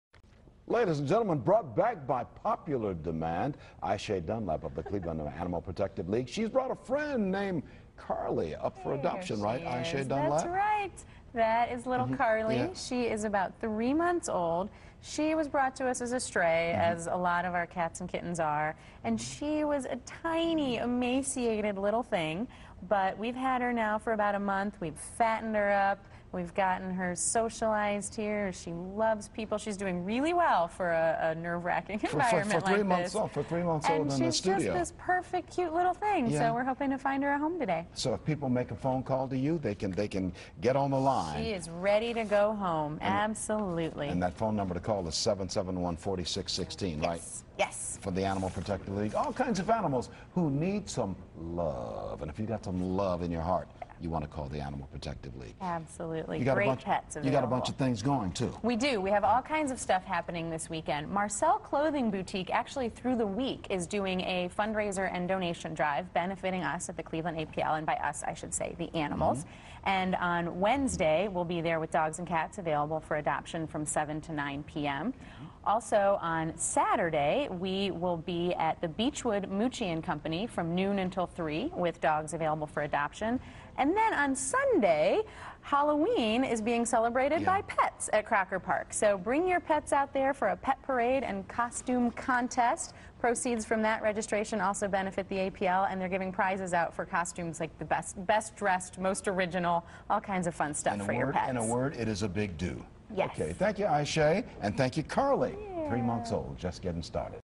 APL Carly
Ayse Dunlap from the Cleveland APL brings a kitten named Carly to the studios.